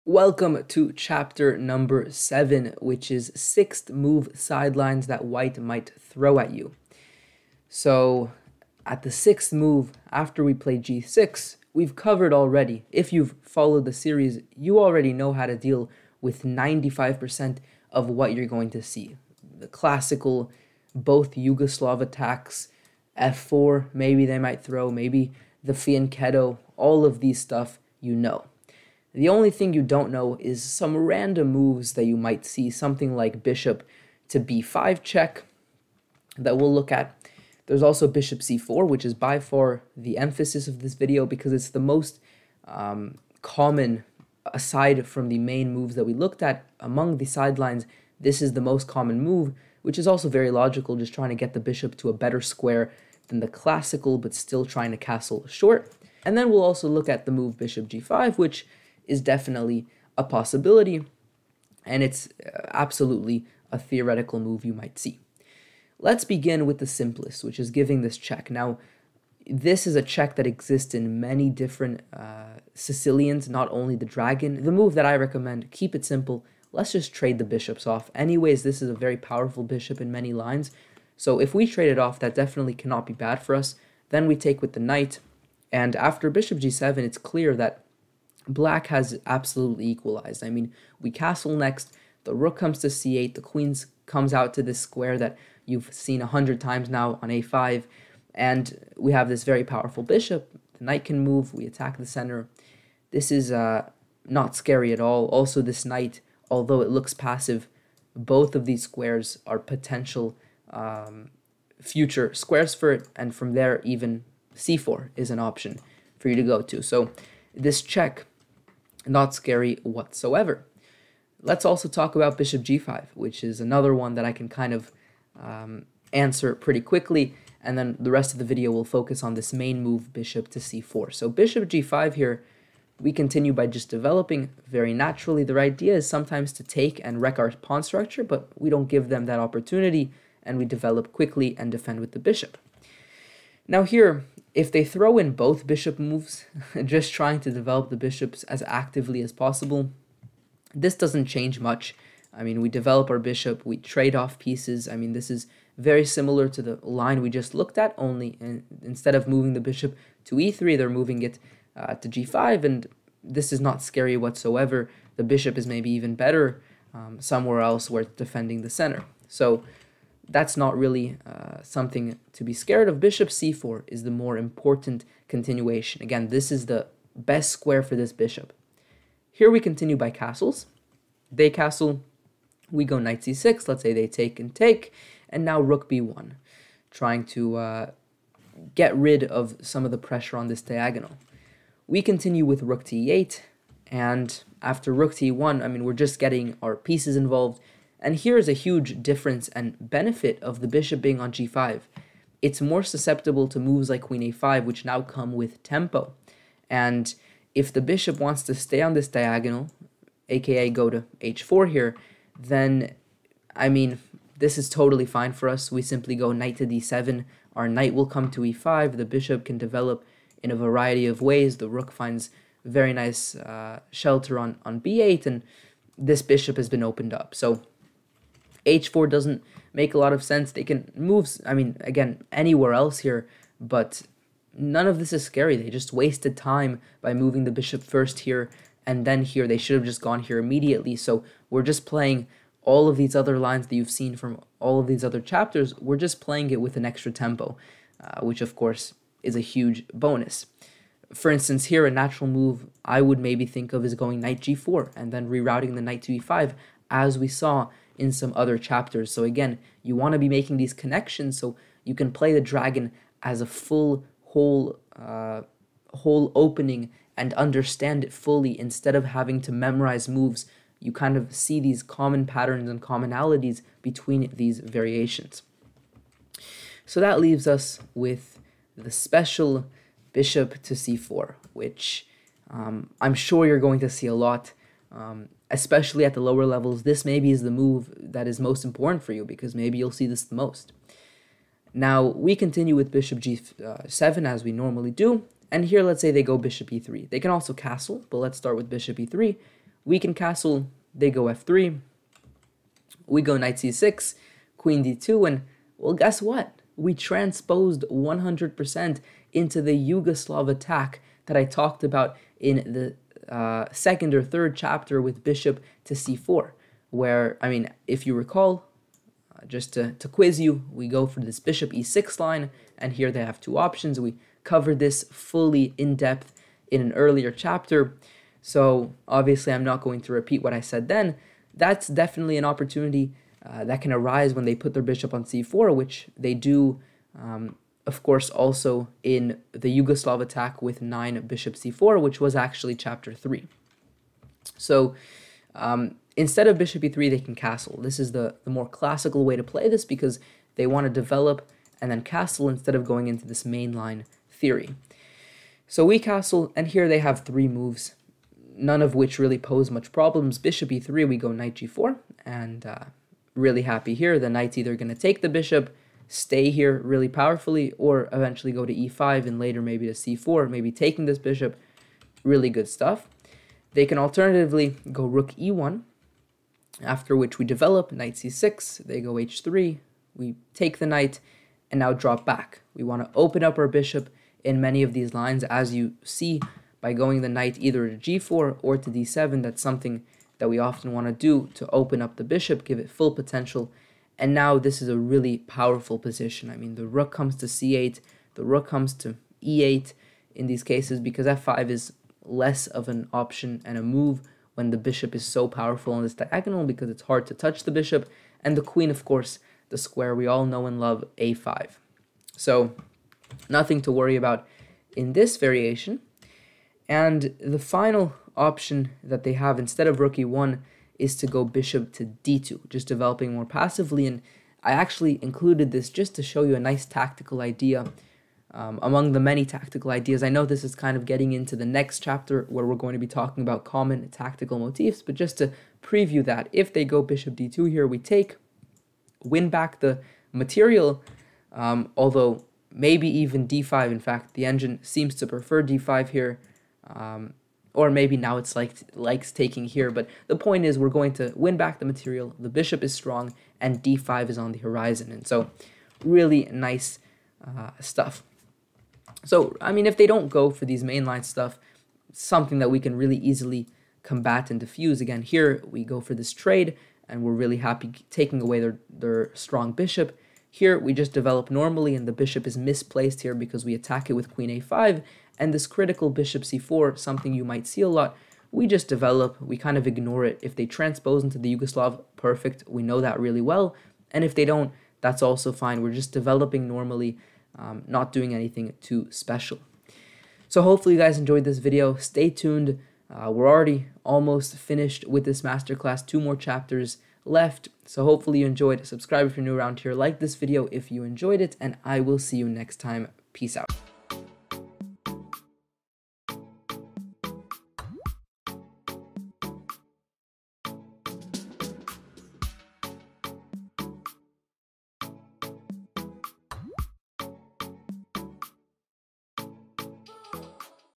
6th Move Sidelines - Sicilian Dragon Masterclass
The Sicilian Dragon is a top-level opening you can play against 1.e4. Instead of a brief explanation of it, I am going in full depth on the Sicilian Dragon Defense. I will include EVERY SINGLE Variation of the Sicilian Defense Dragon Variation opening. This video goes over the 6th Move Sidelines. This is a top pick for white in the Dragon Defense and the 6th Move Sidelines is a slightly theoretical opening you should know. The 6th Move Sidelines are not dangerous but should be known how to be dealt with correctly in the Sicilian Dragon.

Pgn of 6th Move Sidelines:
1. e4 c5 2. Nf3 d6 3. d4 cxd4 4. Nxd4 Nf6 5. Nc3 g6 6. Bc4 (6. Bb5+ Bd7 7. Bxd7+ Nbxd7 8. O-O Bg7) (6. Bg5 Bg7 7. Bb5+ (7. Bc4 O-O 8. O-O Nc6 9. Nxc6 bxc6 10. Rb1 Re8 11. Re1 Qa5 12. Bh4 Nd7) 7... Bd7 8. Bxd7+ Nbxd7) 6... Bg7 7. Be3 ( 7. O-O O-O 8. Be3 (8. Bd2 Nxe4 9. Nxe4 Bxd4) (8. Re1 Nc6 9. h3 Nxd4 10. Qxd4 Nd7 11. Qd2 Ne5 12. Bb3 Bd7) 8... Ng4) 7... O-O 8. f3 Nc6 9. Qd2 Nxd4 10. Bxd4 Be6 *

Chapters:
0:00 Introduction
1:05 6.Bb5+
2:00 6.Bg5
4:36 6.Bc4
7:38: Recap/Overview/Summary